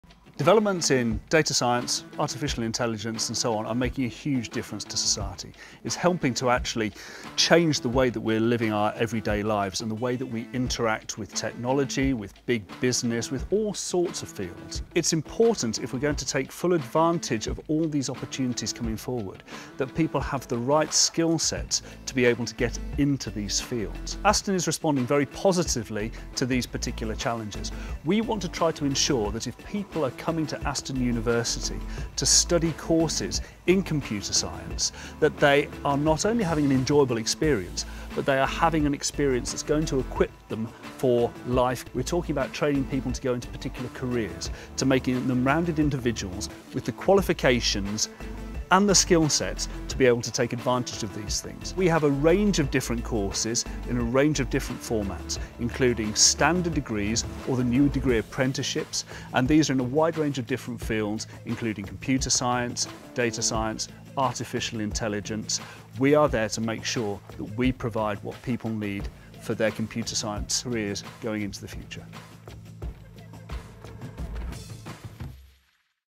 Why Computer Science Matters
What is it about Computer Science that makes it so vital to the world we live in today?